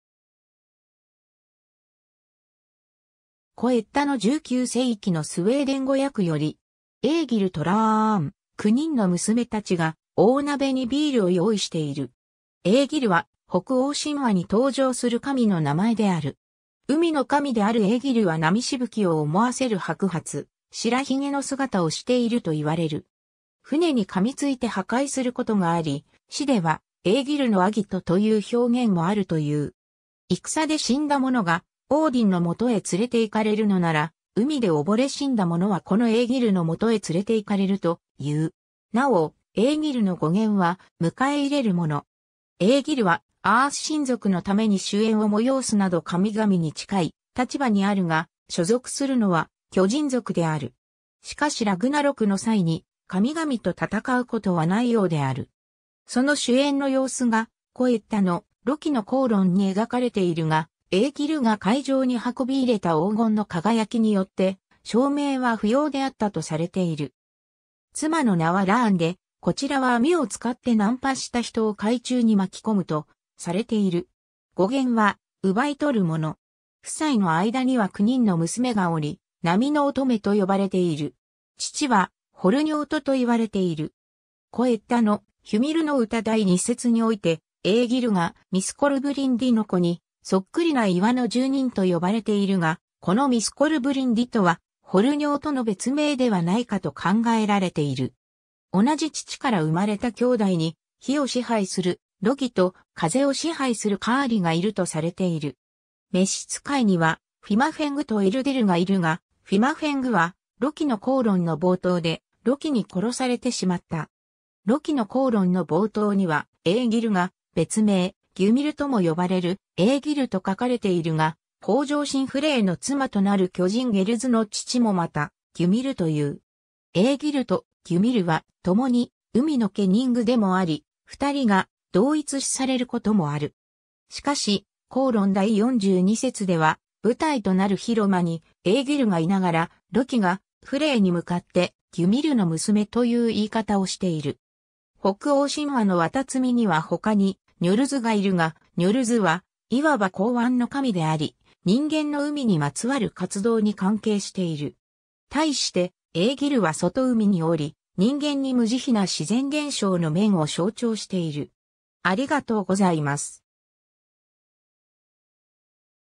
エーギル
『古エッダ』の19世紀のスウェーデン語訳より。
エーギルとラーン、9人の娘たちが、大鍋に麦酒を用意している。
 エーギル（古ノルド語: Ægir）は、北欧神話に登場する神の名前である。
 海の神であるエーギルは 波飛沫を思わせる白髪・白髭の姿をしているといわれる。
 船に噛みついて破壊することがあり、詩では「エーギルのあぎと」という表現もあるという。
 戦で死んだ者がオーディンの元へ連れていかれるのなら、海で溺れ死んだ者はこのエーギルの元へ連れていかれるという。
なお、 エーギルの語源は『迎え入れる者』。
 エーギルはアース神族のために酒宴を催すなど神々に近い立場にあるが、所属するのは巨人族である。
しかしラグナロクの際に神々と戦うことはないようである。
 その酒宴の様子が『古エッダ』の『ロキの口論』に描かれているが、エーギルが会場に運び入れた黄金の輝きによって照明は不要であったとされている。
 妻の名はラーンで、こちらは網を使って難破した人を海中に巻き込むとされている。
 語源は『奪い取る者』。
 夫妻の間には9人の娘がおり、「波の乙女」と呼ばれている。
 父はフォルニョートといわれている。
 『古エッダ』の『ヒュミルの歌』第2節において、エーギルが「ミスコルブリンディの子にそっくりな岩の住人（巨人）」と呼ばれているが、このミスコルブリンディとはフォルニョートの別名ではないかと考えられている。
 同じ父から生まれた兄弟に、火を支配するロギと、風を支配するカーリ（Kári）がいるとされている。
 召使いには、フィマフェングとエルディルがいるが、フィマフェングは『ロキの口論』の冒頭でロキに殺されてしまった。
 『ロキの口論』の冒頭には、エーギルが「別名ギュミルとも呼ばれるエーギル」と書かれているが、豊穣神フレイの妻となる巨人ゲルズの父もまたギュミルという。
「エーギル」と「ギュミル」はともに海のケニングでもあり、2人が同一視されることもある。
しかし、『-口論』第42節では、舞台となる広間にエーギルがいながら、ロキがフレイに向かって「ギュミルの娘」という言い方をしている 。
 北欧神話の海神には他にニョルズがいるが、ニョルズはいわば港湾の神であり、人間の海にまつわる活動に関係している。
対してエーギルは外海におり、人間に無慈悲な自然現象の面を象徴している。